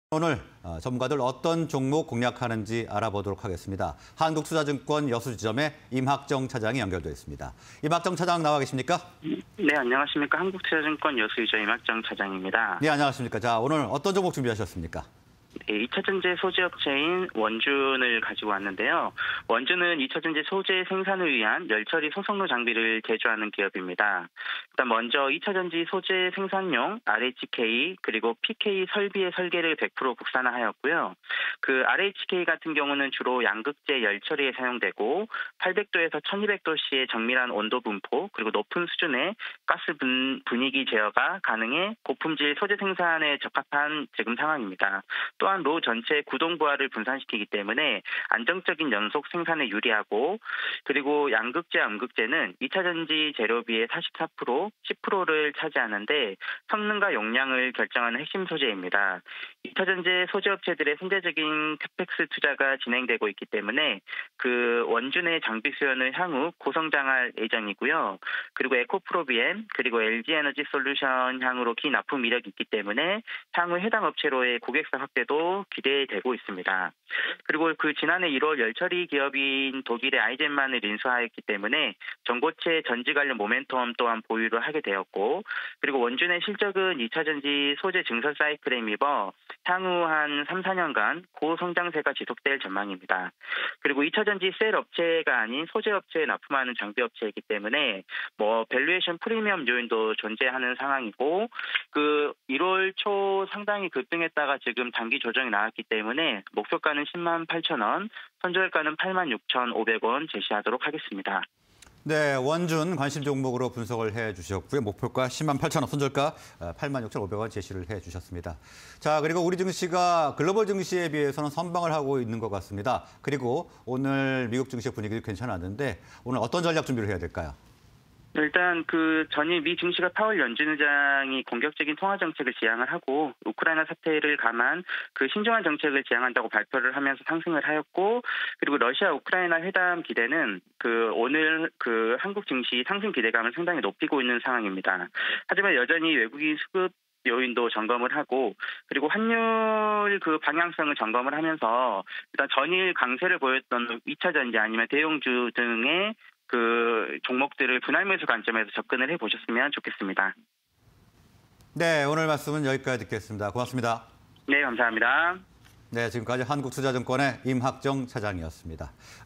원준(382840)... 배터리 소재 증설 트렌드_굿머니 공략주 (20220303)
원준(382840)
- 배터리 소재 증설 트렌드
- 중장기 동사 장비의 수요 증가 예상
- 탄소섬유 소성로 국산화 수혜